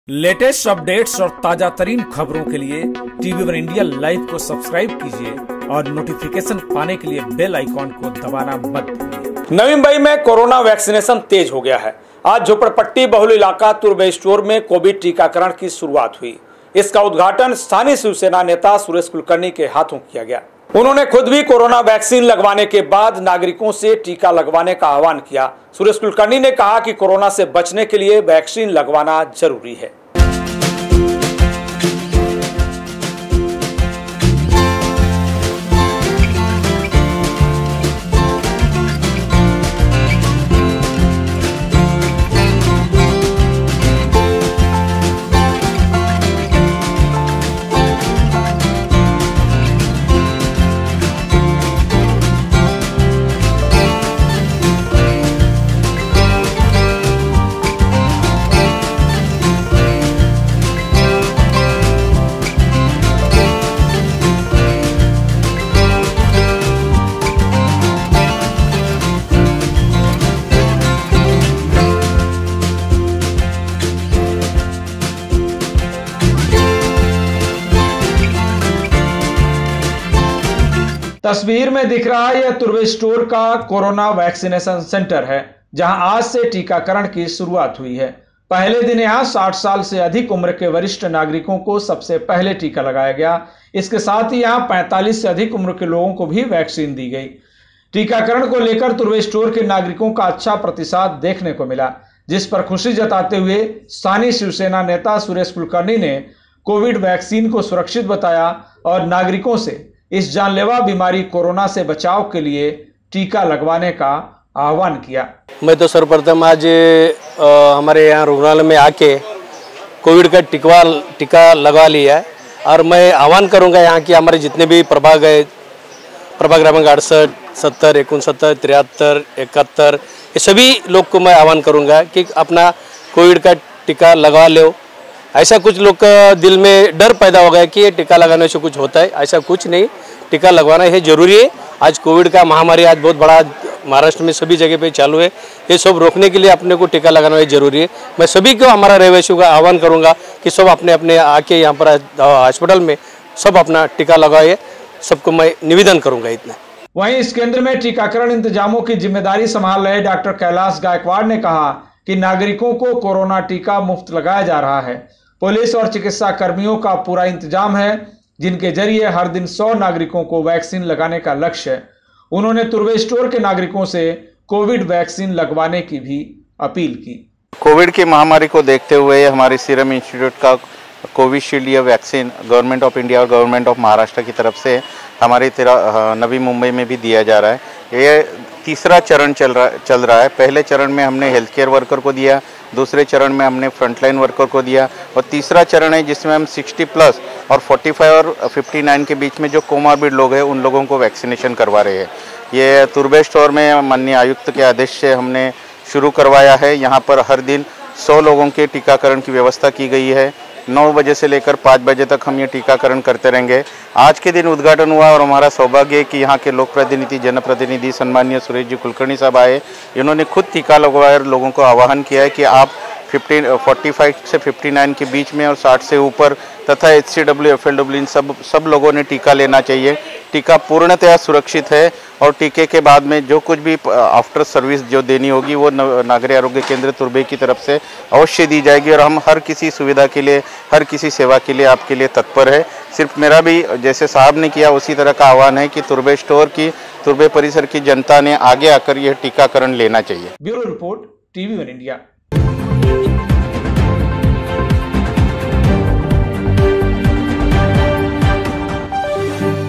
Navi Mumbai के तुर्भे में Corona टीका सेंटर का शुभारंभ | Suresh Kulkarni लगवाया वैक्सिन
NaviMumbai के तुर्भे में CoronaVaccination टीका सेंटर का शुभारंभ | SureshKulkarni लगवाया वैक्सिन
नवी मुंबई में कोरोना वैक्सिनेशन तेज हो गया है. आज झोपड़पट्टी बहुल इलाका तुर्भे स्टोर्स में कोविड टीकाकरण की शुरूआत हुई.सबसे पहले यहां 60 साल से अधिक उम्र के बुजुर्गों के लिए टीकाकरण हुआ. तस्वीर में दिख रहे इस केन्द्र से टीकाकरण का शुभारंभ स्थानीय शिवसेना नेता सुरेश कुलकर्णी के हाथों हुआ.